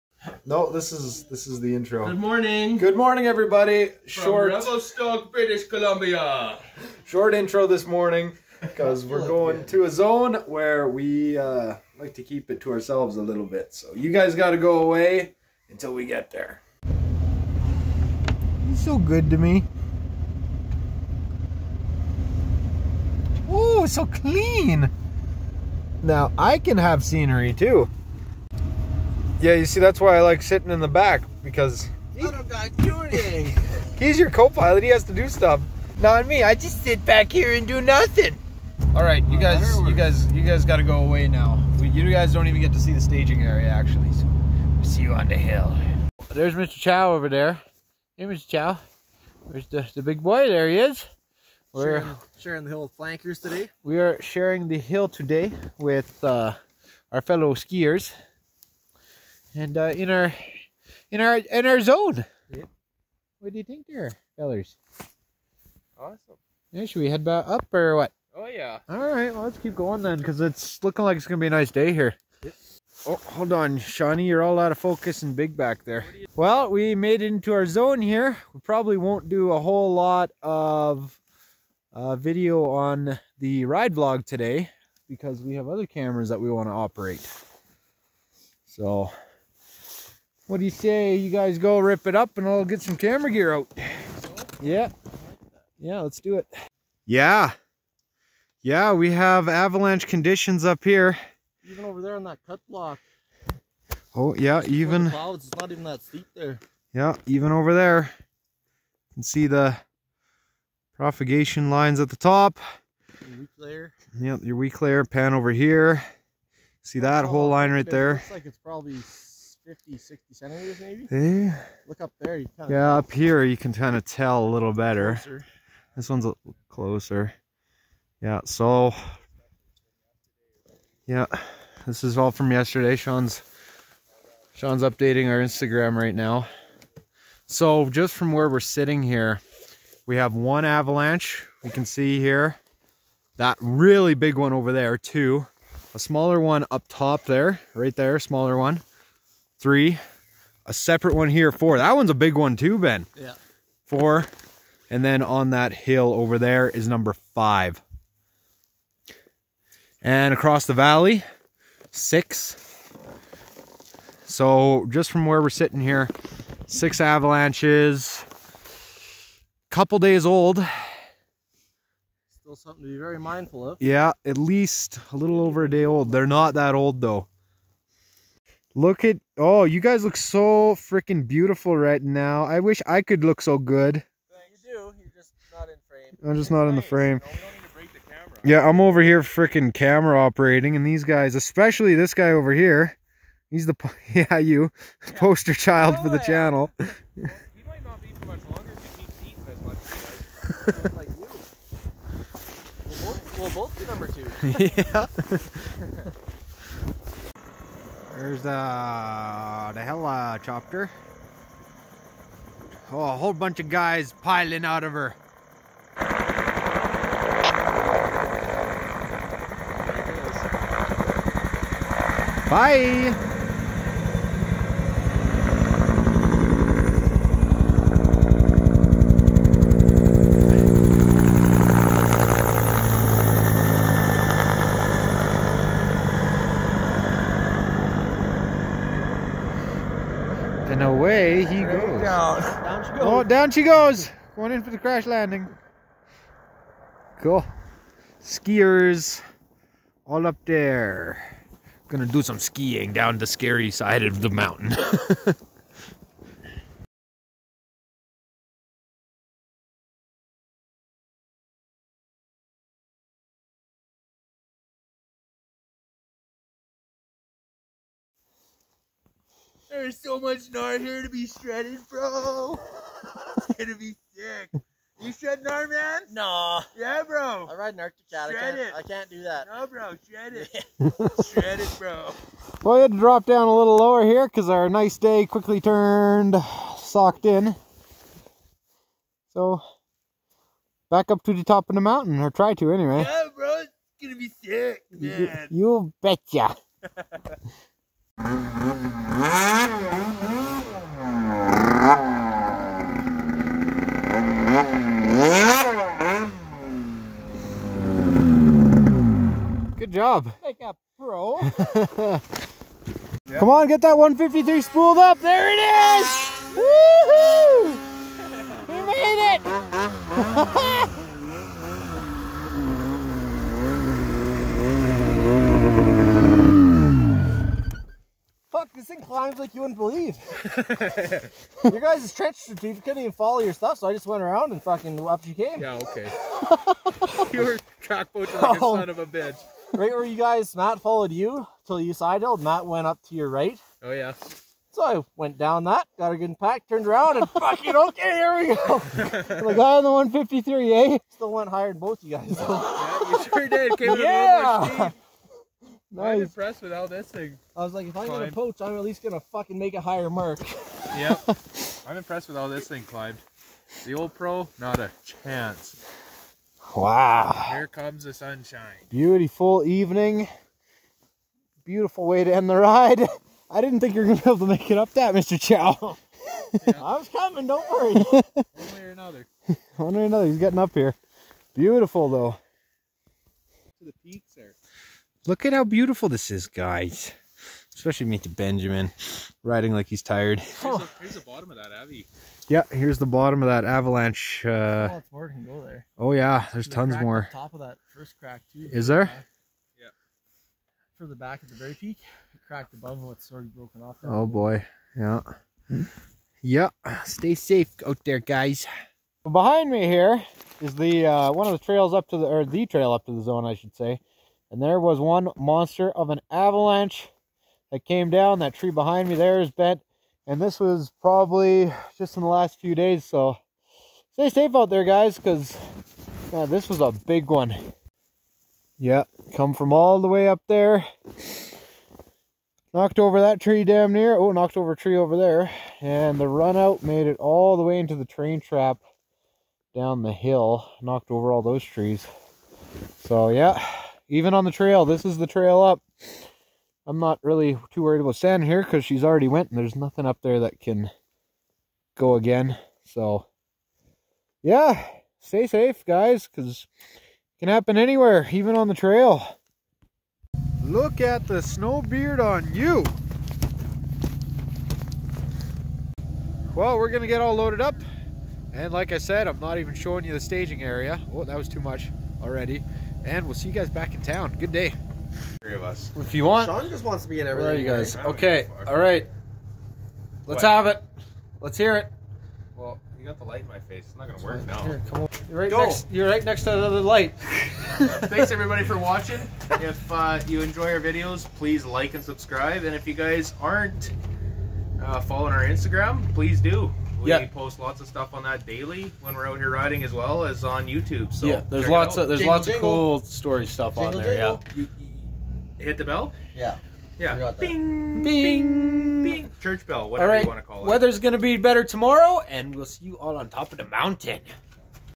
The DEEPEST snow we can Find
A more secretive zone. Heading up to find some of that steep and deep!